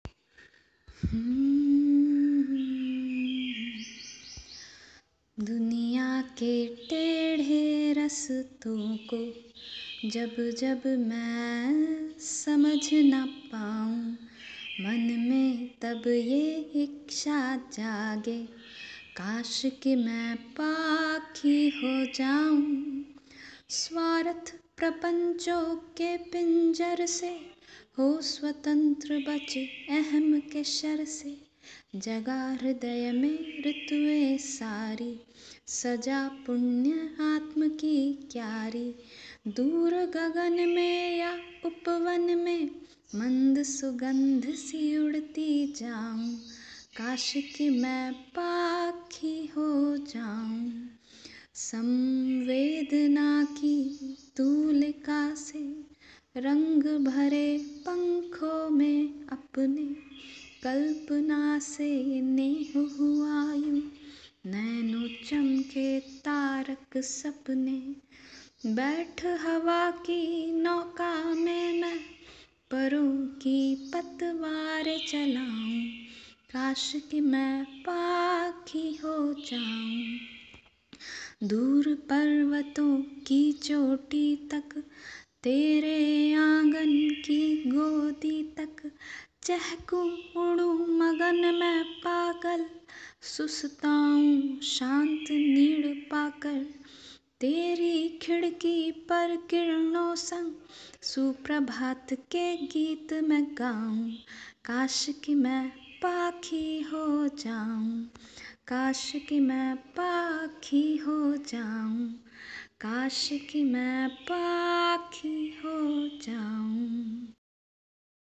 Kaash Ki Main Paakhi Ho Jaun | काश की मैं पाखी हो जॉन | Hindi Kavita | हिंदी कविता
"The human bird shall take his first flight, filling the world with amazement, all writings with his fame, and bringing eternal glory to the nest whence he sprang" - Leonardo da Vinci

"Be as a bird perched on a frail branch that she feels bending beneath her, still she sings away all the same, knowing she has wings" - Victor Hugo

"Delicious autumn! My very soul is wedded to it, and if I were a bird I would fly about the earth seeking the successive autumns" - George Eliot

Poem written and recited by Sugandha Agnihotri
Visuals arranged by Channel moderator
Credits for images - PixaLab